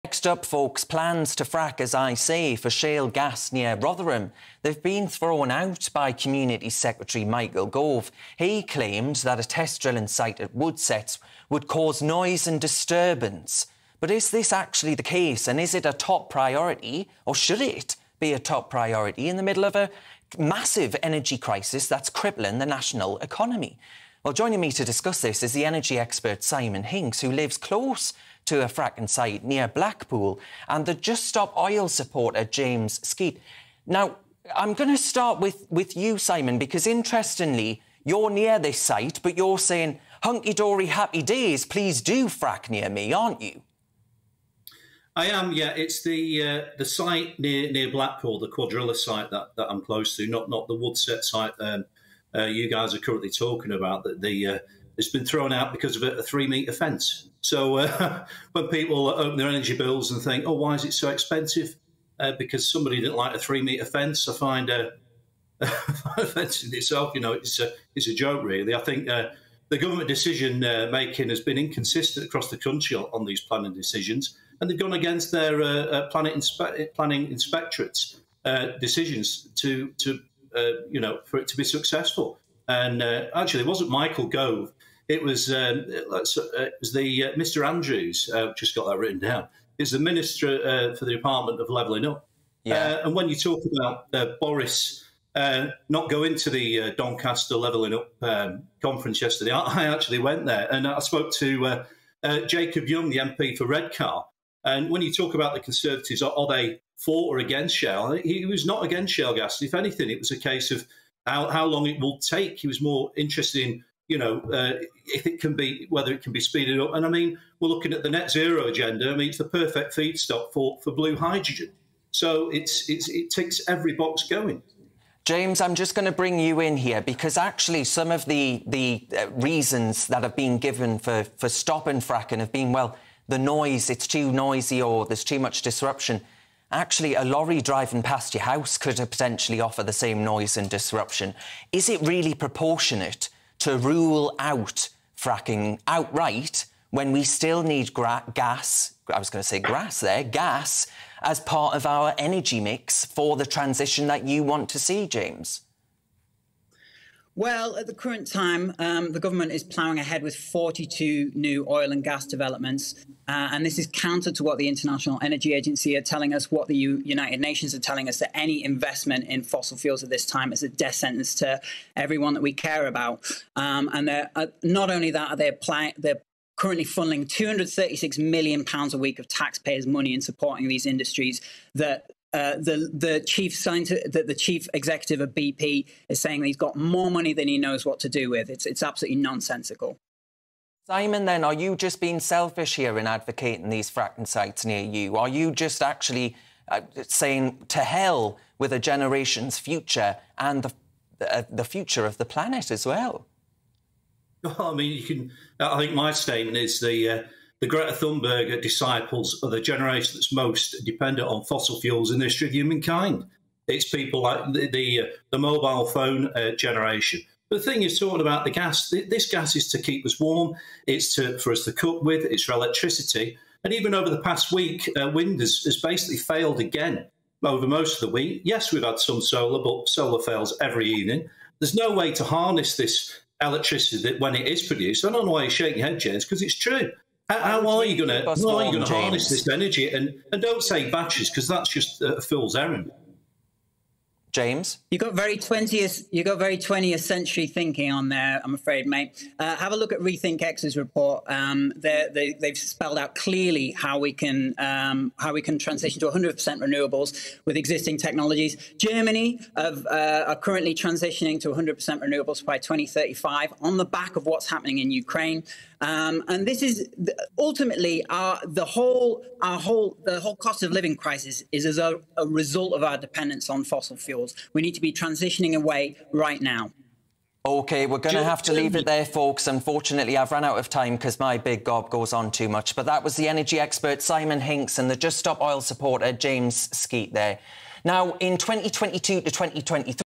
James Skeet | GB News | 18 June 2022 | Just Stop Oil
COP26 has failed: there are no meaningful commitments to halt fossil fuel production, our future now a reckless gamble.